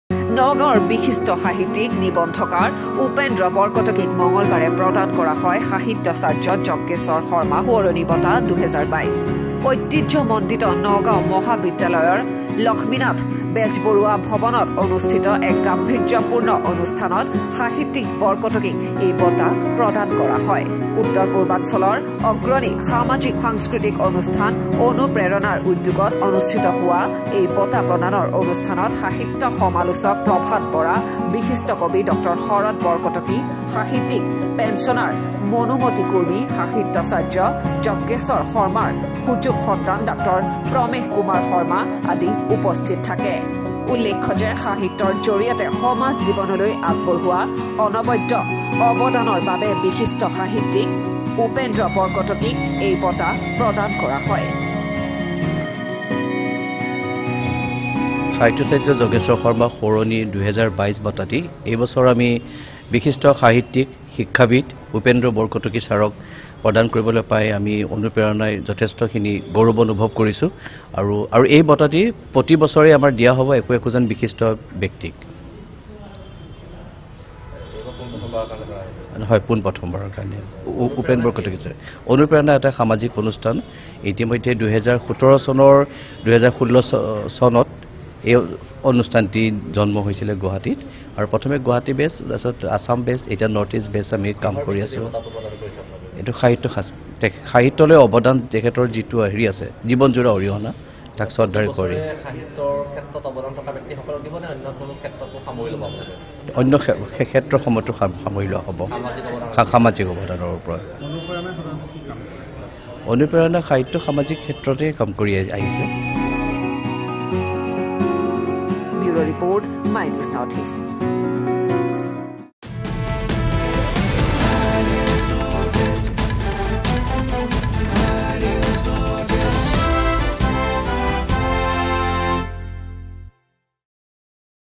🔹বিশিষ্ট সাহিত্যিক উপেন্দ্ৰ বৰকটকীক সাহিত্যচাৰ্য যজ্ঞেশ্বৰ শৰ্মা সোঁৱৰণি বঁটা...
My News NE:  🔹বিশিষ্ট সাহিত্যিক উপেন্দ্ৰ বৰকটকীক সাহিত্যচাৰ্য যজ্ঞেশ্বৰ শৰ্মা সোঁৱৰণি বঁটা...